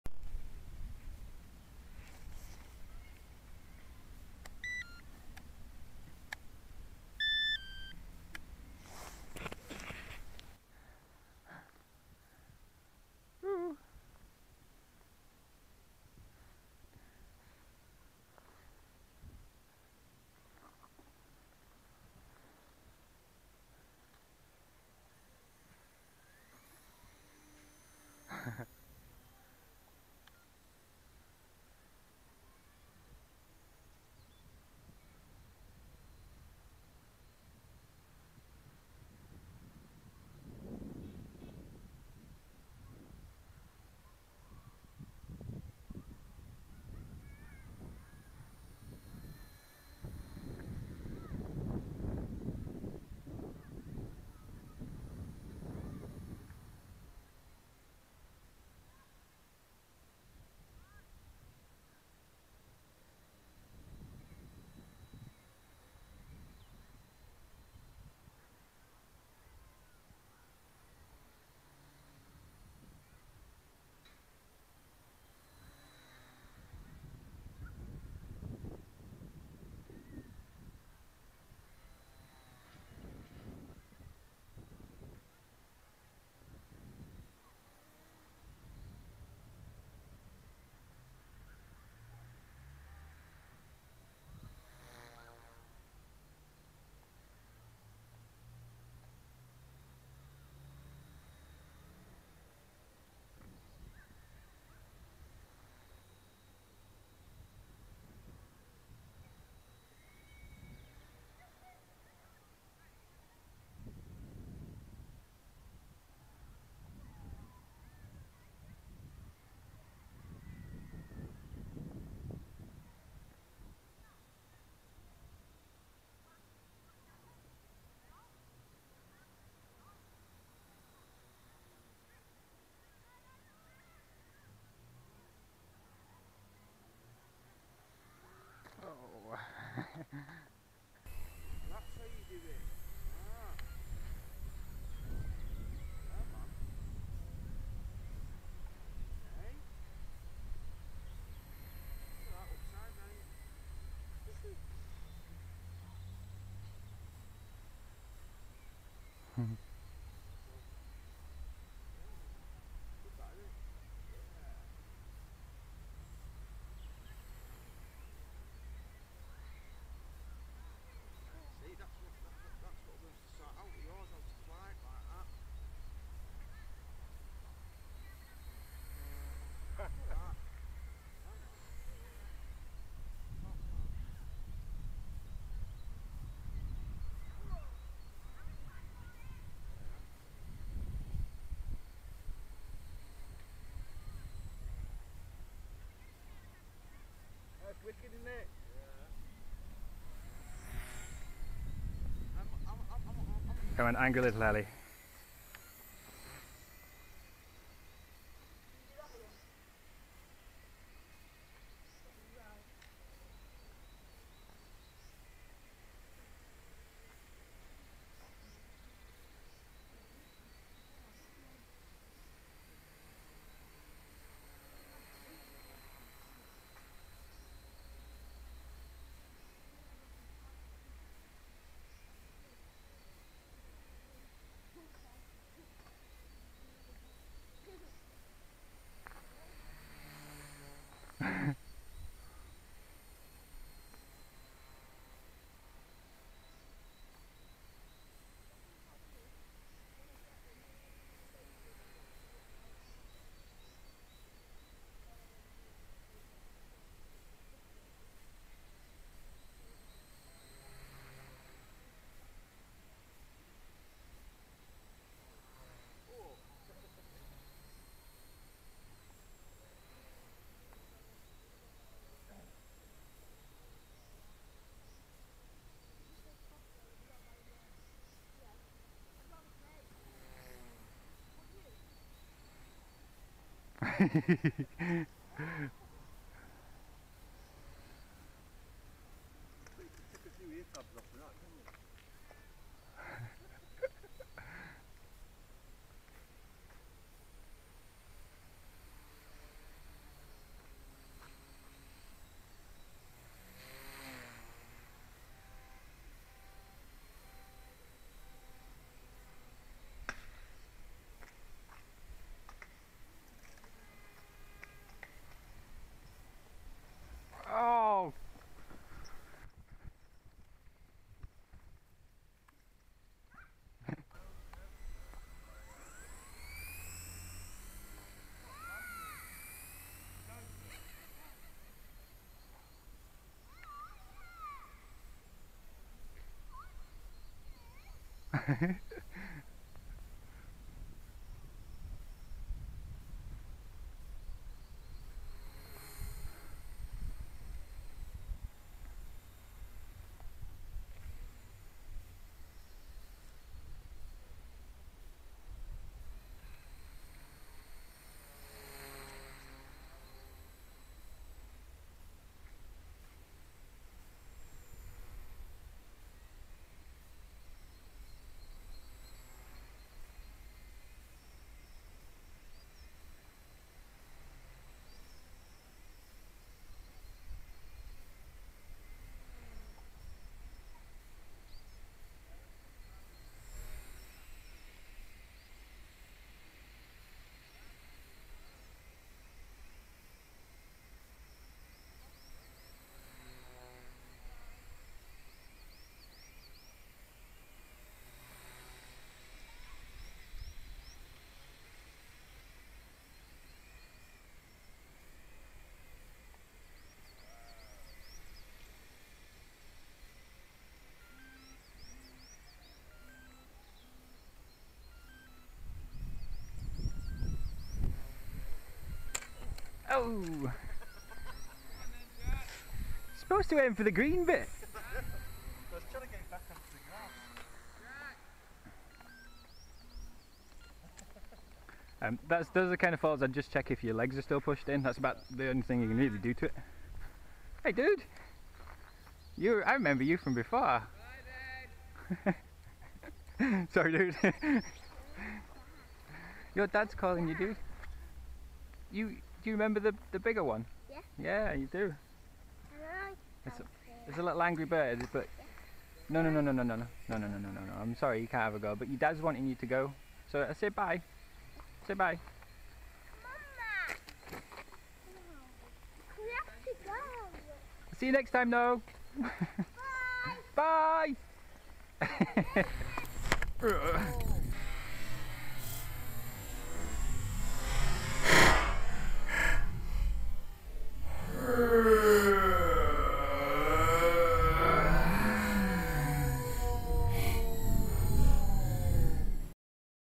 Super CP - tail first inverted practice
New favourite groove is inverted tail slides. I really like the bullet blades for circuits, its feels like I can bank the heli further than stock blades and the sounds is excellent. Just need some bright orange ones.

Good weekend for flying.